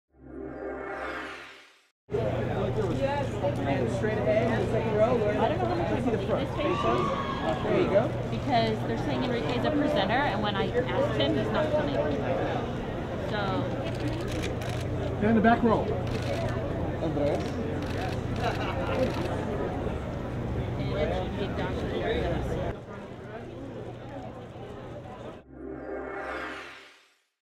Andrea Sixtos | 2014 NCRL ALMA Awards | Red Carpet
2014 NCLR ALMA Awards Red Carpet Arrivals at Pasadena Civic Auditorium in Pasadena, Ca USA October 10, 2014